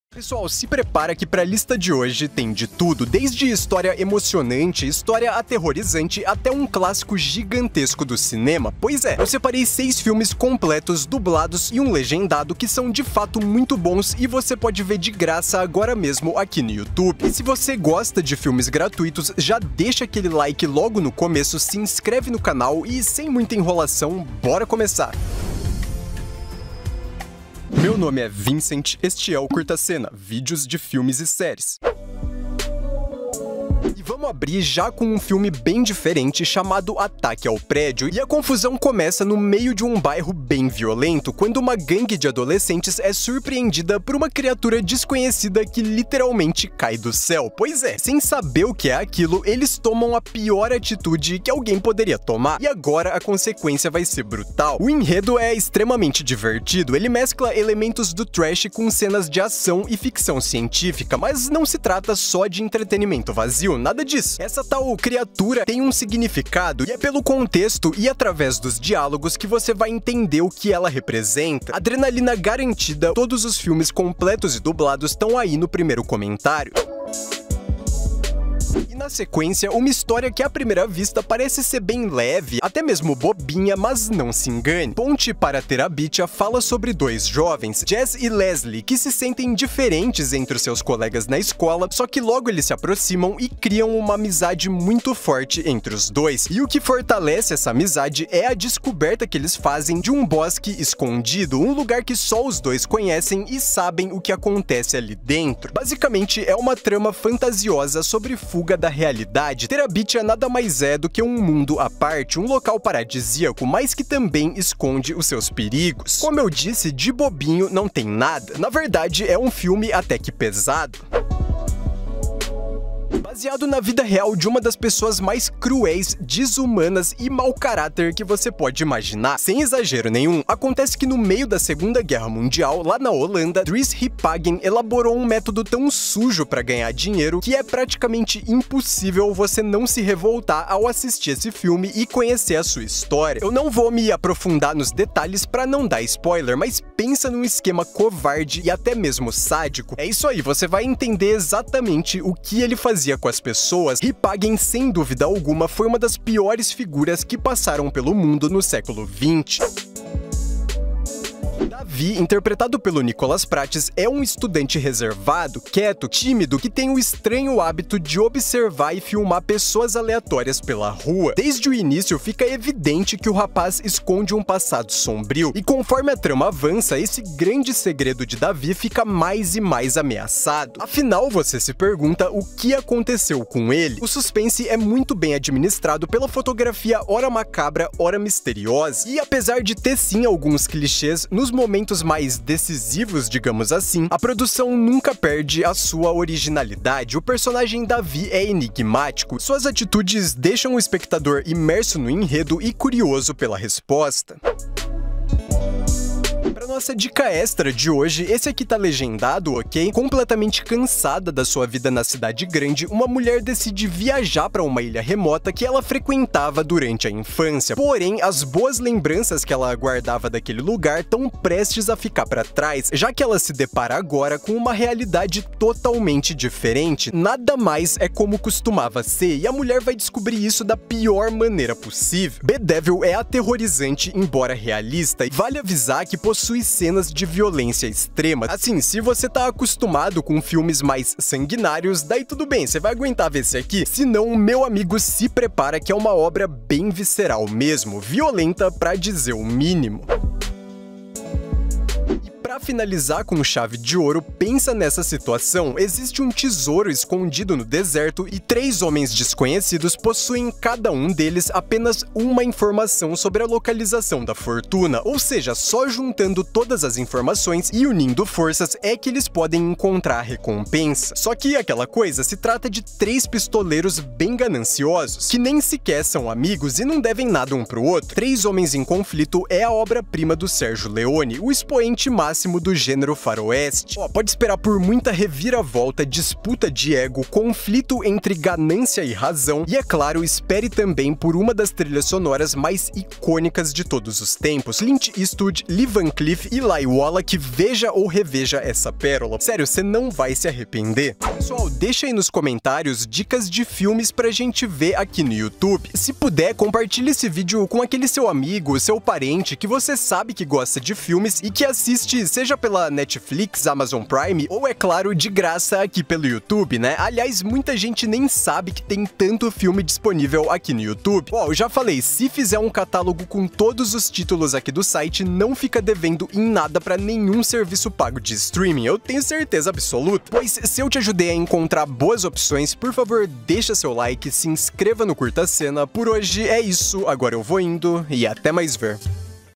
6 FILMES BONS DEMAIS no YOUTUBE | filmes completos dublados 2021
6 bons filmes completos dublados em 2021 no YouTube! | filmes completos de graça

Se prepara que para a lista de hoje tem de tudo: desde história emocionante, história aterrorizante, até um clássico gigantesco do cinema.
Eu separei 6 filmes completos, dublados (e um legendado) que são de fato muito bons e você pode ver de graça aqui no YouTube.

Veja também:
6 FILMES INCRÍVEIS no YOUTUBE DE GRAÇA

OS LINKS PARA OS FILMES ESTÃO NO PRIMEIRO COMENTÁRIO.

Buscas: filmes completos dublados hd; filmes em português; melhores filmes YouTube.